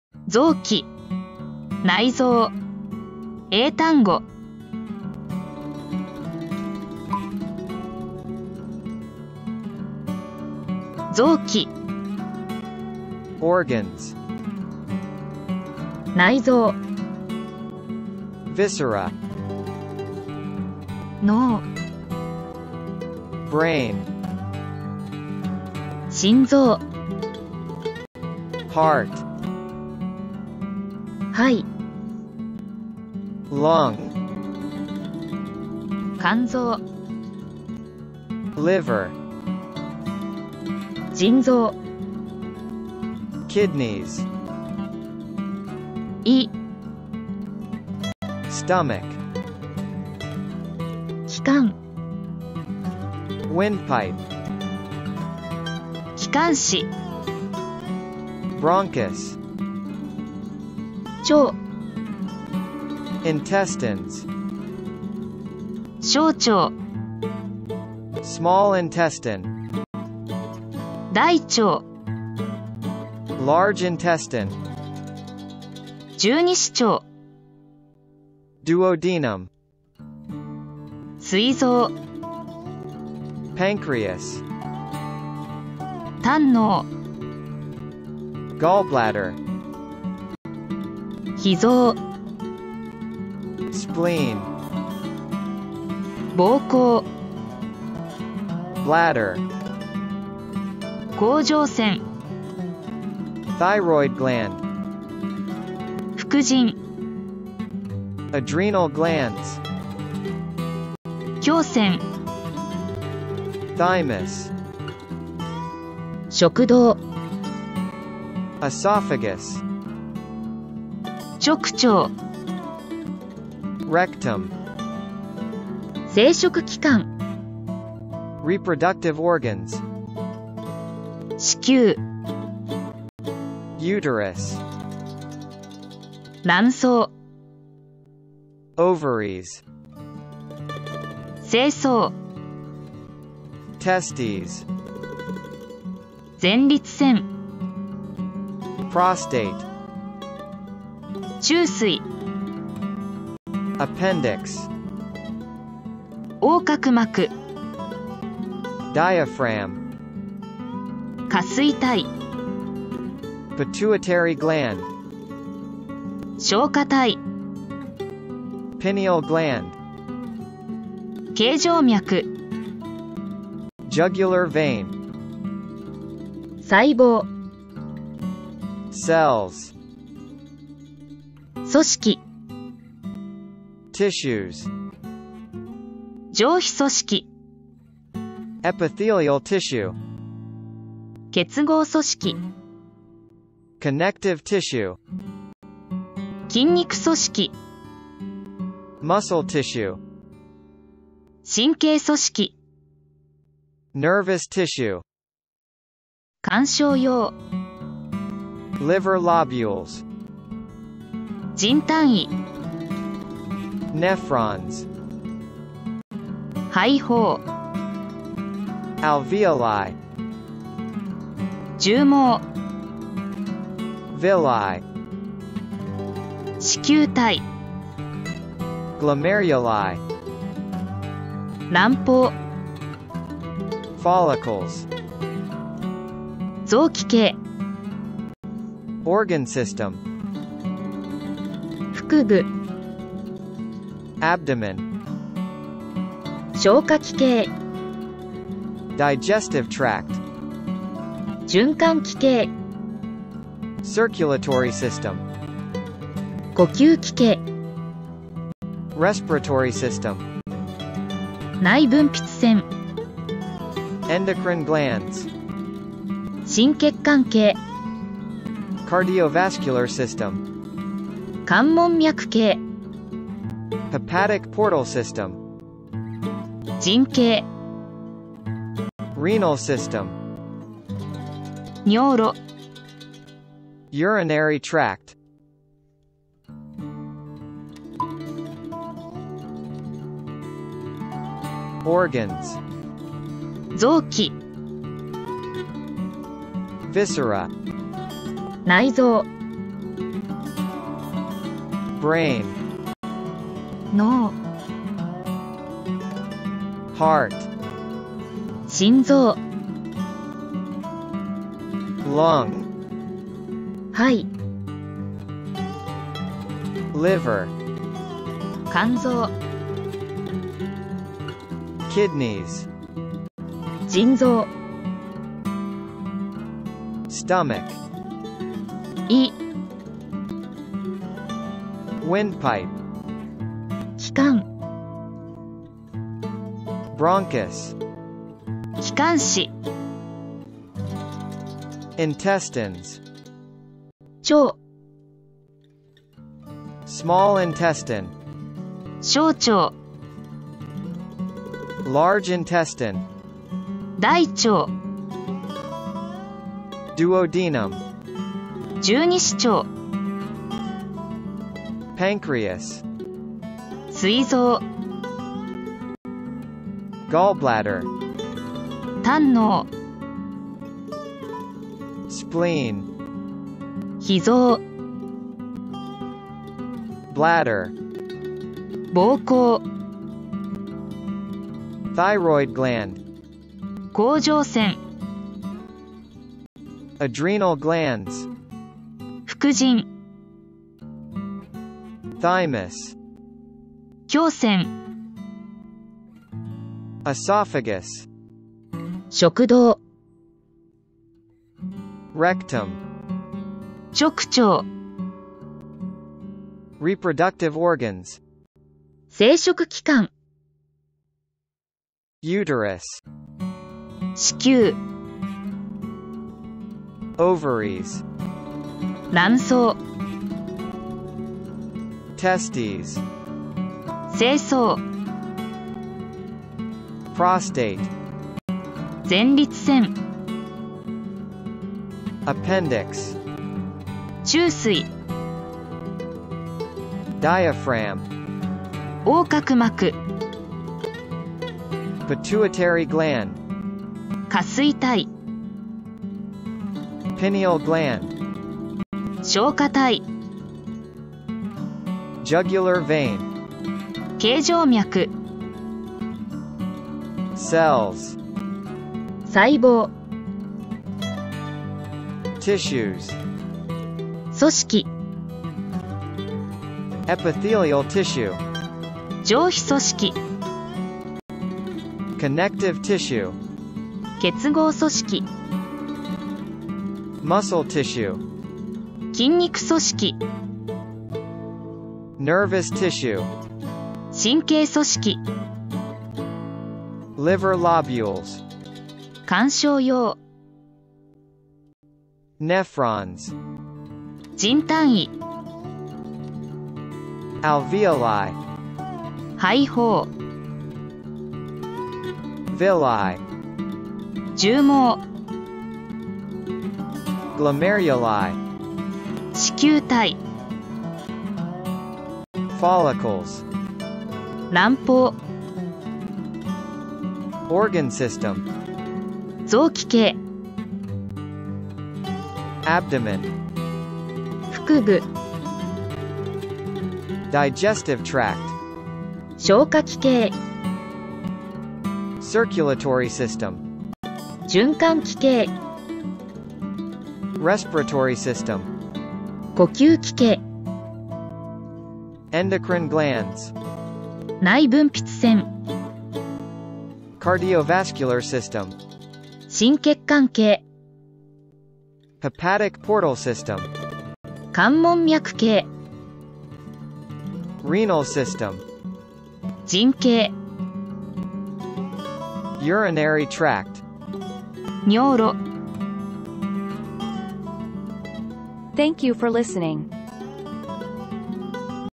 【55単語】内臓・臓器 英単語 (English Words for Organs and Viscera)
＊＊＊＊＊動画内では以下の単語を紹介しています（一部抜粋）＊＊＊＊＊
1. organs：臓器
2. viscera：内臓
3. brain：脳
4. heart：心臓
5. lung：肺
6. liver：肝臓
7. kidneys：腎臓
8. stomach：胃
9. windpipe：気管
10. bronchus：気管支
11. intestines：腸
12. small intestine：小腸
13. large intestine：大腸
14. duodenum：十二指腸
15. pancreas：膵臓
16. gallbladder：胆嚢
17. spleen：脾臓
18. bladder：膀胱
19. thyroid gland：甲状腺
20. adrenal glands：副腎

前半は日本語→英語、後半は英語→日本語　の順番に音声が流れます。

0:00:00　オープニング
0:00:05　前半(日本語→英語)
0:04:45　後半(英語→日本語)

実際の状況によっては別の言い回しの方が適切な場合もあると思うので、適宜ご確認いただけると幸いです。 一般的に馴染みのある言葉から、専門性の高い単語まで広くカバーできるよう、今度も動画作成していきたいと思います。どうぞよろしくお願いいたします。